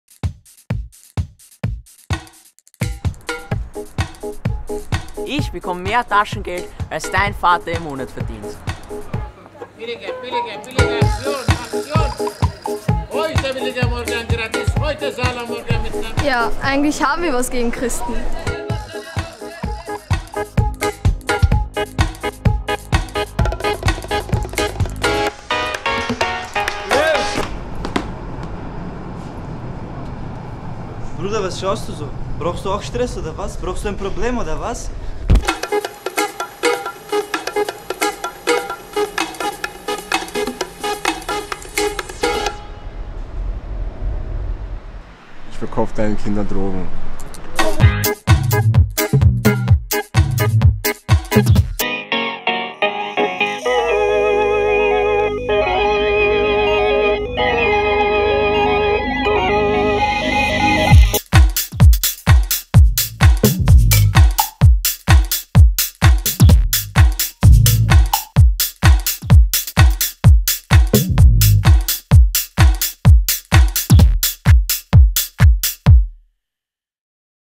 ANTIRASSISMUS-SPOT "...Glauben Sie wirklich?"
Anti-racism spot made by austrian students.

Shot in one day with available light in Vienna, Austria.
Total expense: 85€ :-)

Will be shown at the Austrian "THIS HUMAN WORLD" human-rights-filmfestival.